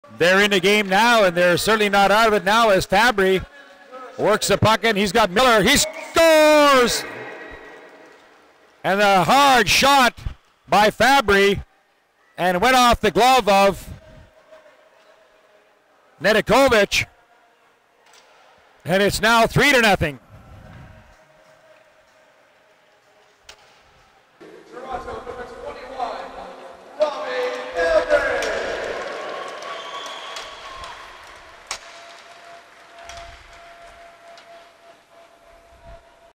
Robby Fabbri - 2006 - Brick Invitational Hockey Tournament
Robby Fabbri #21 at the Brick Super Novice Hockey Tournament at West Edmonton Mall in Edmonton, Alberta, Canada.

This video is of Robby Fabbri when he was 9 or 10 years old, depending on the clip!

Toronto Pro Great Goal and 1st Star